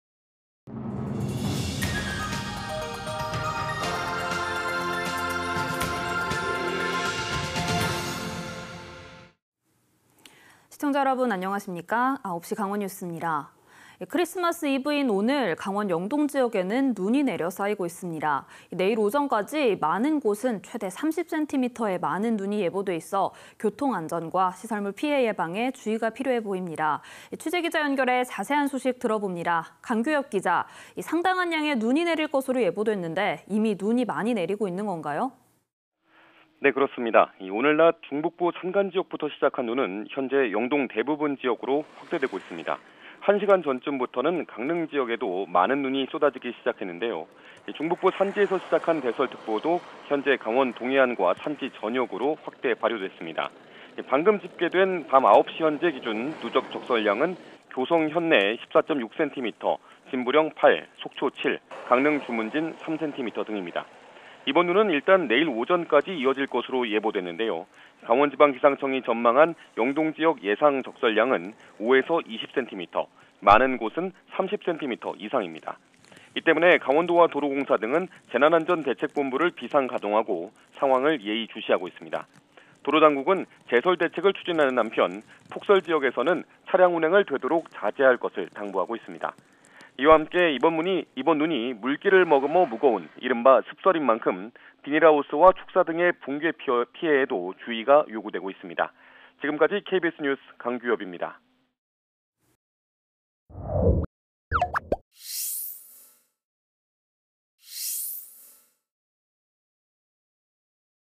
성탄전야 영동 폭설…최대 30cm 예보 / KBS 2021.12.24.
[앵커]

 크리스마스이브인 오늘(24일) 강원 영동지역에는 눈이 내려 쌓이고 있습니다.

 내일 오전까지  많은 곳은 최대 30센티미터의  많은 눈이 예보돼있어, 교통 안전과 시설물 피해 예방에 주의가 필요해 보입니다.

 취재 기자 연결해 자세한 소식 들어봅니다.

 강규엽 기자!

 상당한 양의 눈이 내릴 것으로 예보됐는데, 이미 눈이 많이 내리고 있는 건가요?

 [리포트]

 오늘 낮 중북부 산간지역부터 시작한 눈은 현재 영동 대부분 지역으로 확대되고 있습니다.

 한 시간 전쯤부터는 강릉지역에도 많은 눈이 쏟아지기 시작했는데요.

 중북부 산지에서 시작한 대설특보도 현재 강원 동해안과 산지 전역으로 확대 발효됐습니다.

 방금 집계된 밤 9시 현재 기준 누적 적설량은 고성 현내 14.6센티미터, 진부령 8, 속초 7, 강릉 주문진 3센티미터 등입니다.

 이번 눈은 일단 내일 오전까지  이어질 것으로 예보됐는데요.

 강원지방기상청이 전망한 영동지역 예상 적설량은 5~20센티미터, 많은 곳은 30센티미터 이상입니다.

 이 때문에  강원도와 도로공사 등은 재난안전대책본부를 비상 가동하고, 상황을 예의 주시하고 있습니다.

 도로 당국은 제설 대책을 추진하는 한편, 폭설 지역에서는 차량 운행을 되도록 자제할 것을 당부하고 있습니다.

 이와 함께 이번 눈이 물기를 머금어 무거운 이른바 '습설'인 만큼,  비닐하우스와 축사 등의 붕괴 피해에도 주의가 요구되고 있습니다.

 KBS 뉴스 강규엽입니다.

 촬영기자:김중용/영상편집:정용진